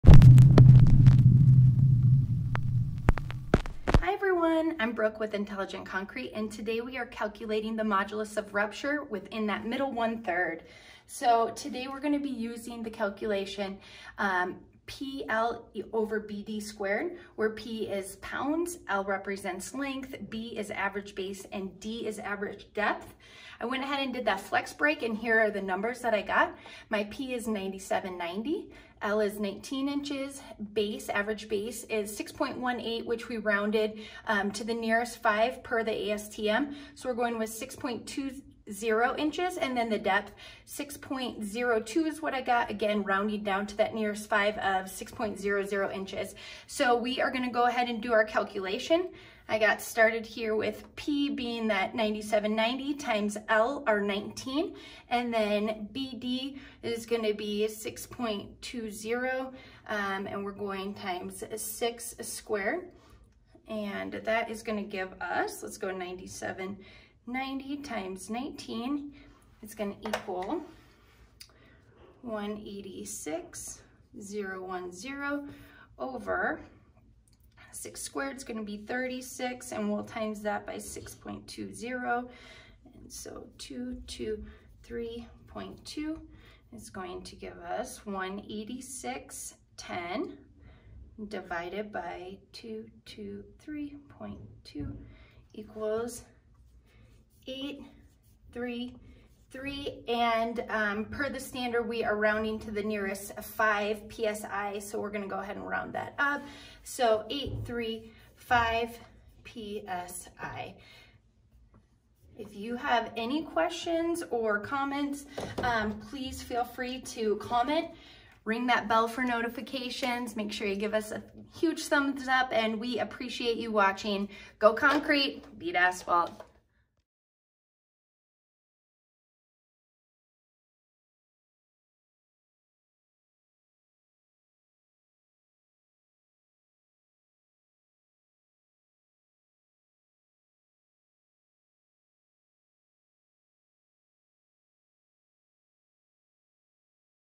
How to Calculate Concrete's Modulus of Rupture: Middle 1/3 of the Beam - Vlog 685
Today Intelligent Concrete's lab technician, Brooke shows you the math that goes into calculating the Modulus of Rupture for a concrete mix after running ASTM C78 for flexural strength. This video covers what calculation to perform if the beam breaks in the middle one third, but check out the next video to see what to do if that's not the case.

CONCRETE MERCH!